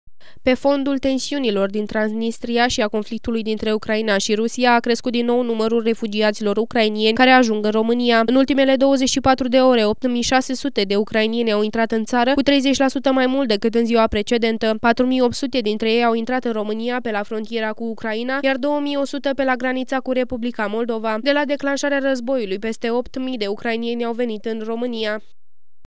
Numărul refugiaților, în creștere la granițe
Pe fondul tensiunilor din Transnistria și a conflictului dintre Ucraina și Rusia a crescut din nou numarul refugiaților ucraineni care ajung în România.